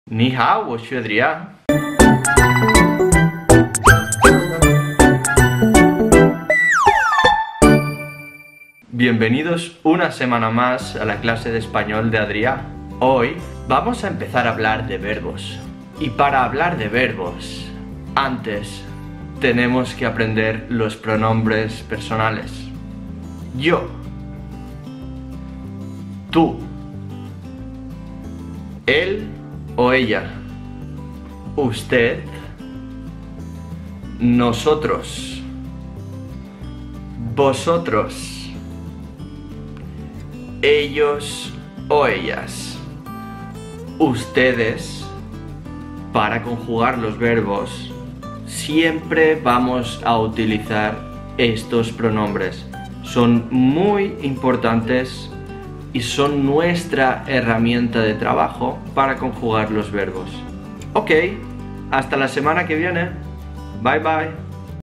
一分鐘介紹西文人稱代名詞｜阿弟仔每週西文教室｜島男西文
人稱代名詞是西文入門必備之一，大家就一起來跟著島男唸吧!